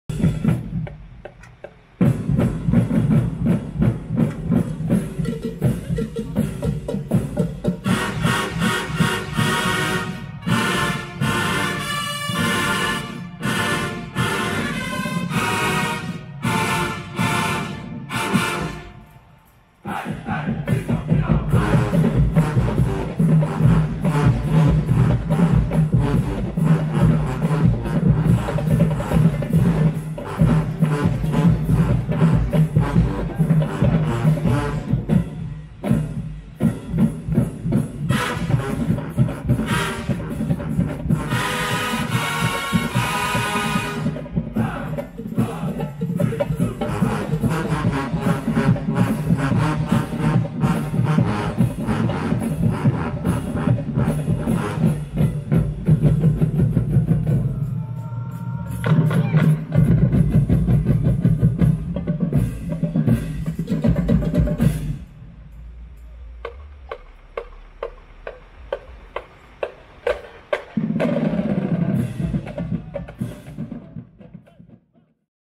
BETHUNE COOKMAN UNIVERSITY MARCHING WILDCATS DANCE ROUTINE
bcu practicing for a show during the spring semester.
I used the DJI pocket and was shocked at how good the audio turned out.